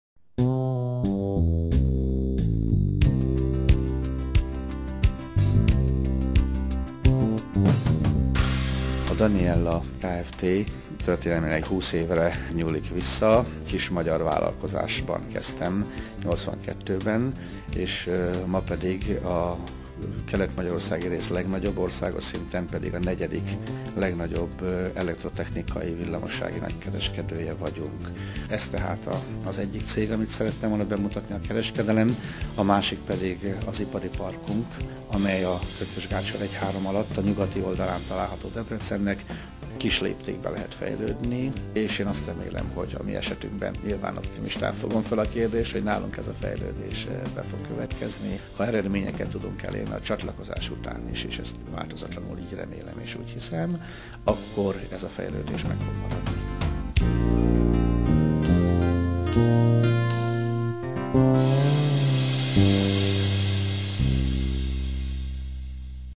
Daniella Kereskedelmi Kft. - Kőszegi Dániel Ügyvezető Igazgató
- Nyilatkozik a(z) Daniella Kereskedelmi CÉG CÉGVEZETŐJE Kőszegi Dániel Ügyvezető Igazgató! Daniella Kereskedelmi Kft. A LEGJELENTŐSEBB MAGYAR CÉGEK KÖZÖTT!